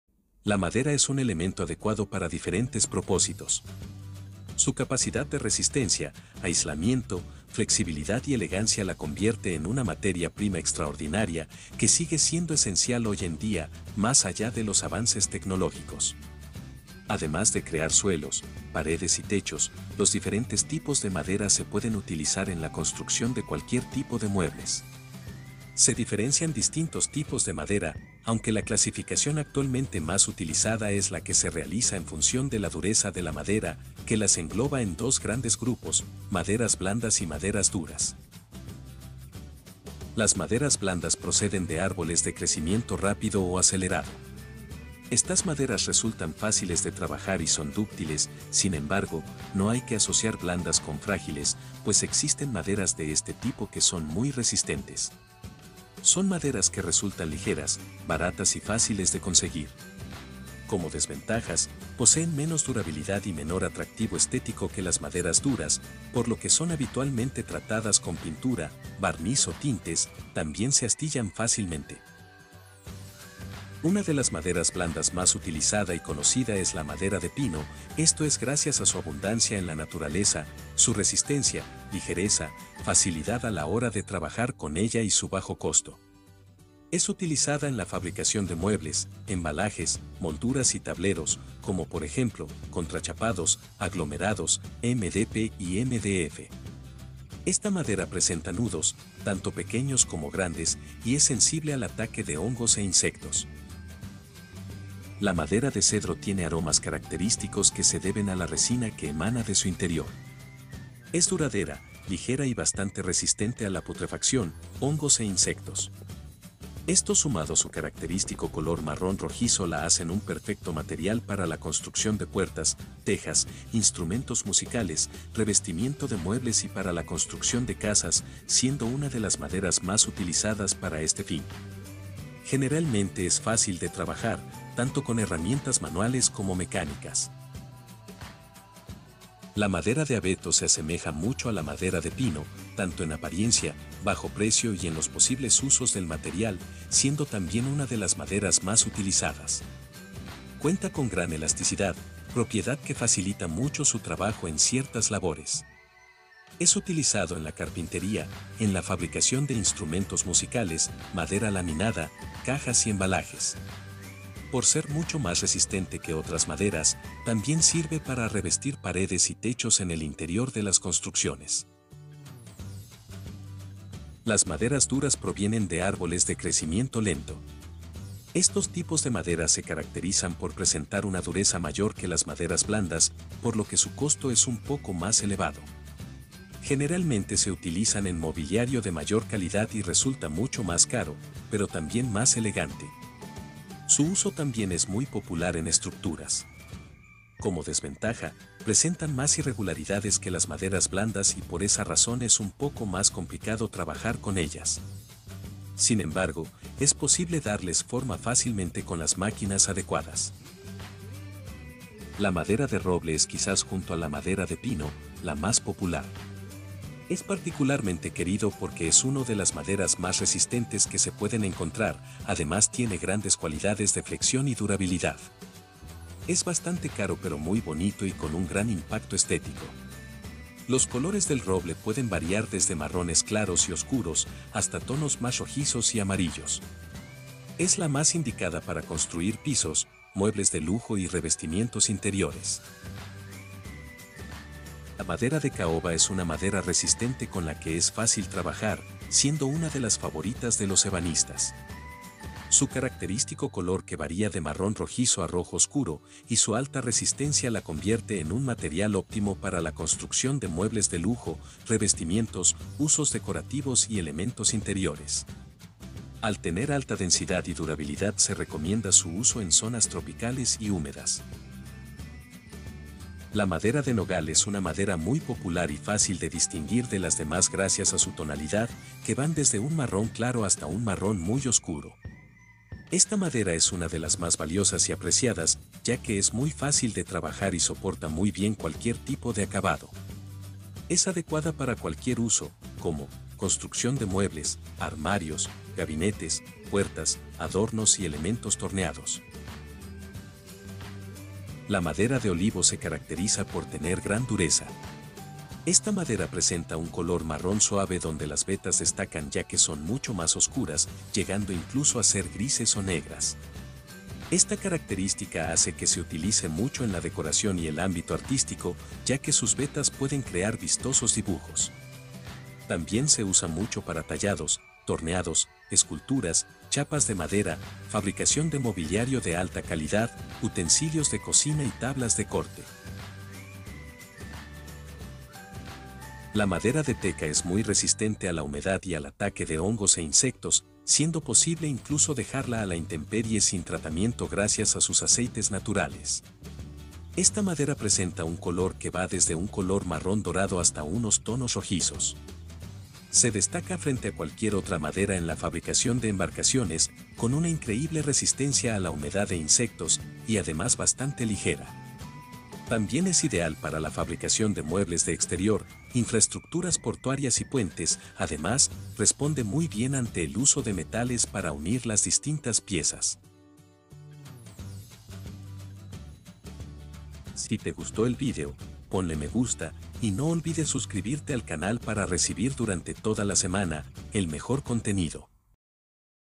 TIPOS DE MADERAS NATURALES SEGÚN SU DUREZA - CARACTERÍSTICAS Y USOS
Maderas duras y maderas blandas más utilizadas.

Capítulos:
00:00 Introducción a los tipos de maderas
00:39 Maderas blandas
01:13 Madera de pino
01:44 Madera de cedro
02:19 Madera de abeto
02:53 Maderas duras
03:32 Madera de roble
04:04 Madera de caoba
04:33 Madera de nogal
05:00 Madera de olivo
05:35 Madera de teca

Diseño de árboles: Hum3D